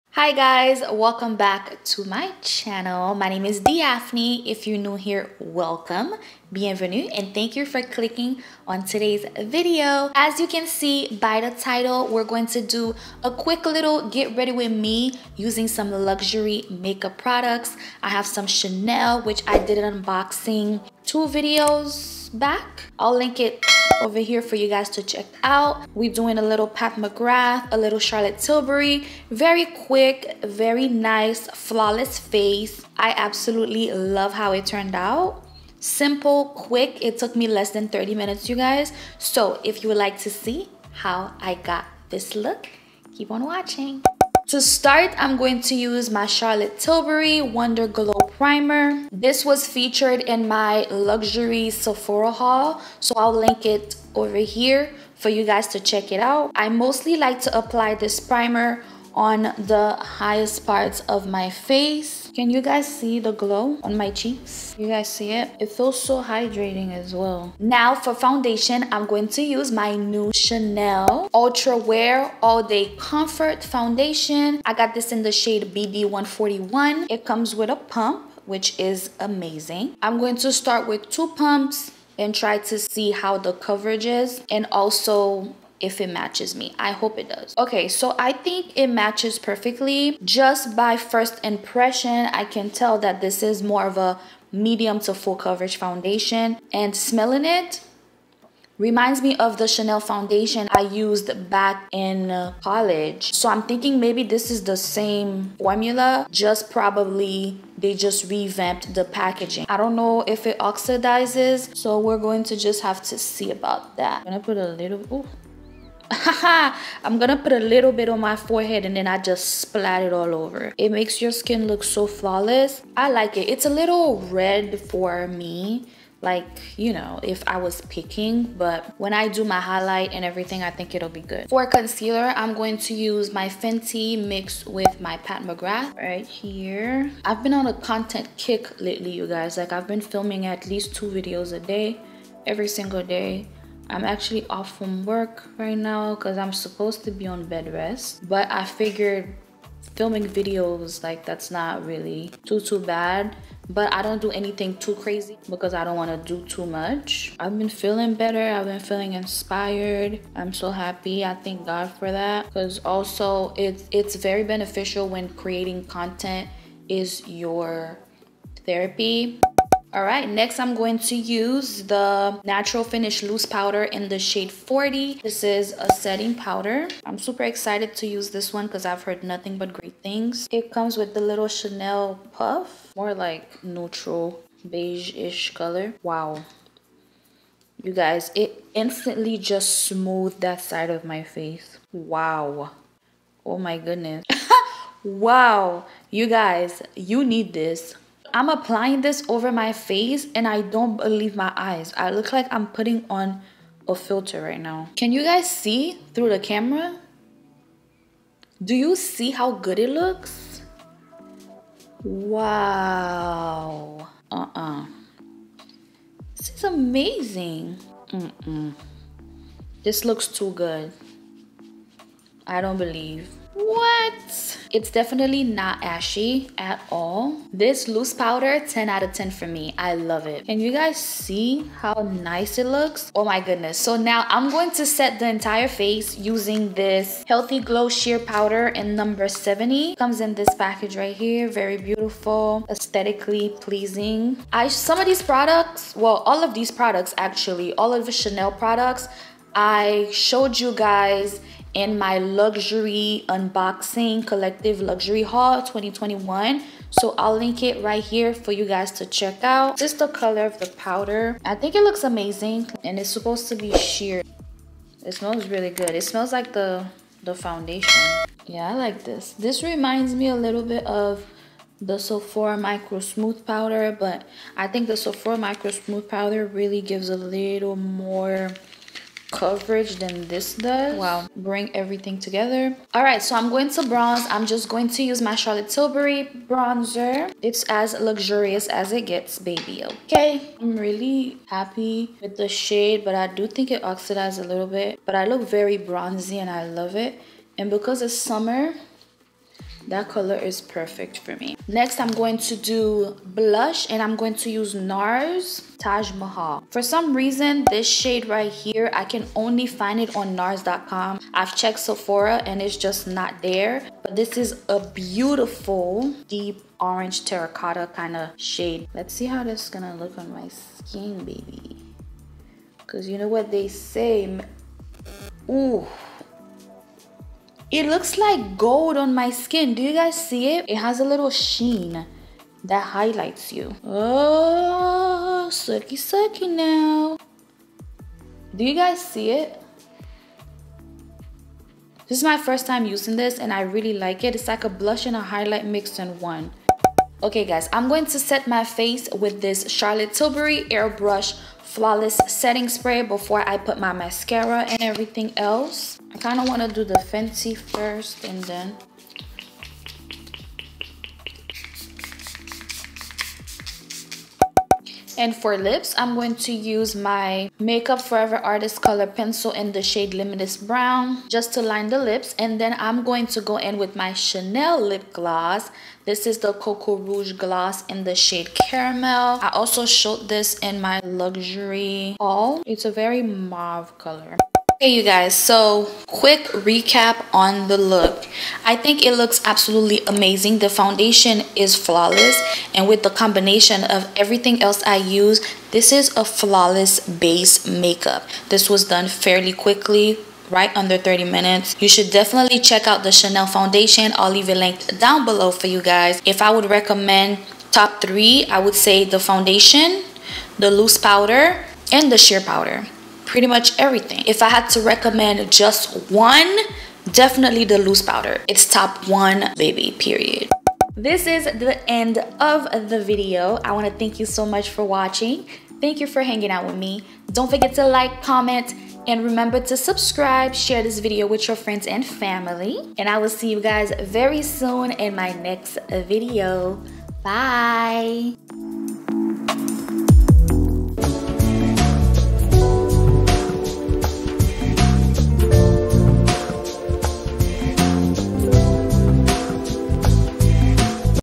Luxury GRWM W. Chanel, Charlotte Tilbury + More | Diaphnie Casimir
♡ Hi, My Loves! Today's Video Is A Luxury + Bougie GRWM Using My New Chanel Makeup From My Collective Luxury Haul And Some More Luxury Makeup Such As Pat McGrath, Charlotte Tilbury And A Little Fenty Beauty Sprinkled In There. I Hope You Enjoy. We're ON The Road To 1,000 Subbies

Mentions ☟

⇢ Cream Shirt - Express (Old Buy)
⇢ Dior Montaigne Earrings
⇢ David Yurman Gold Bracelet
⇢ David Yurman Classic Cable Bracelet

﹆ Face
⇢ Charlotte Tilbury Glow Face Primer
⇢ Chanel UltraWear Comfort Flawless Foundation BD141
⇢ Fenty Beauty Concealer
⇢ Pat McGrath Sublime Perfection Concealer
⇢ Chanel Natural Finish Loose Powder 40
⇢ Chanel Healthy Glow Sheer Setting Powder 70
⇢ Charlotte Tilbury Matte Bronzer Deep
⇢ Nars Blush Taj Mahal

﹆ Lips
⇢ Limitless Brown Pencil Makeup Forever
⇢ Chanel Coco Rouge Gloss IN Caramel

﹆ Eyes (Not Seen IN This Video)
⇢ Better Than Sex Mascara Too Faced
⇢ Fenty Beauty Snap Eyeshadow Fiya Freak ON Lids

Get A FREE Stock From Robinhood
⇢ Join Robinhood with my link and we'll both get free stock 🤝

God First, Always
Phil 4:13​​​ ❥